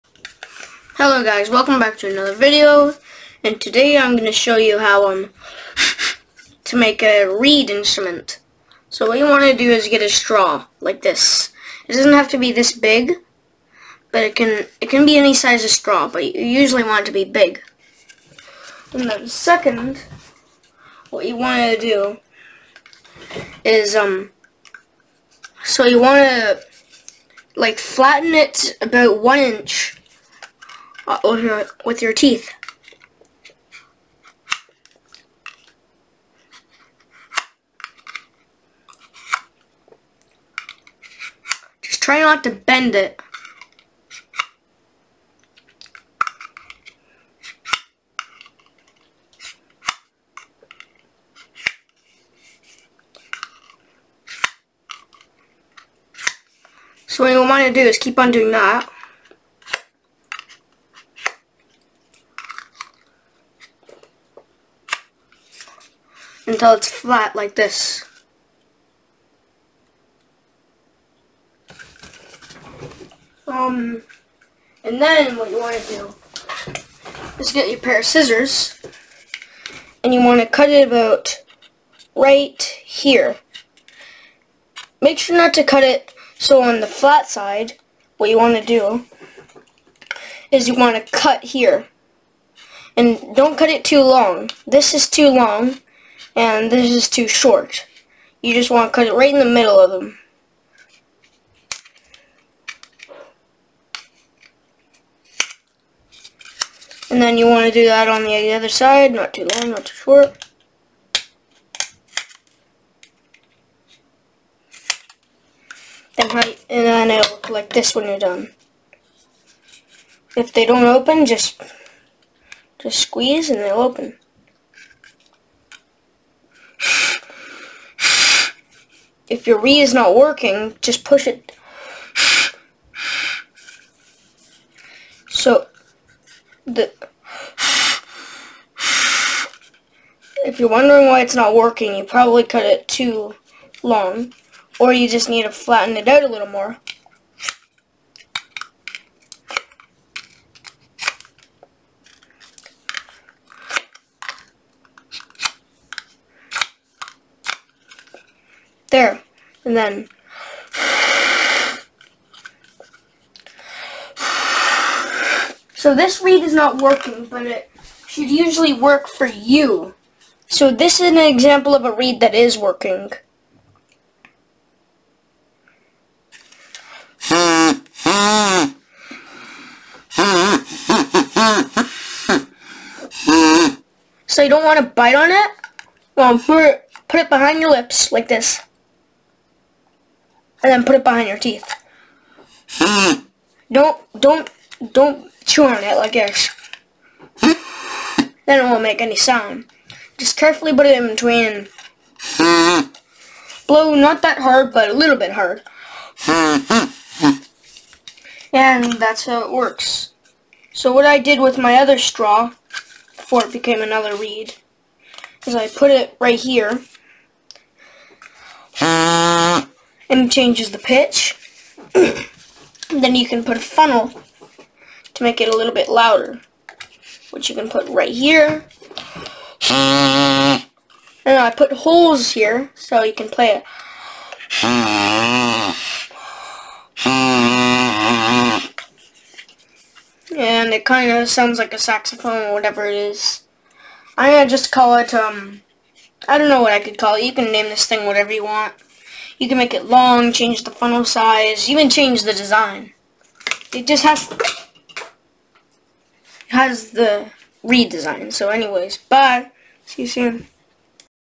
How to make a DIY reed instrument (easy)
DIY reed instrument, using a straw. I am not becoming one of those fake,weird,confusing and downright dangerous DIY channels like 5 Minute Crafts etc.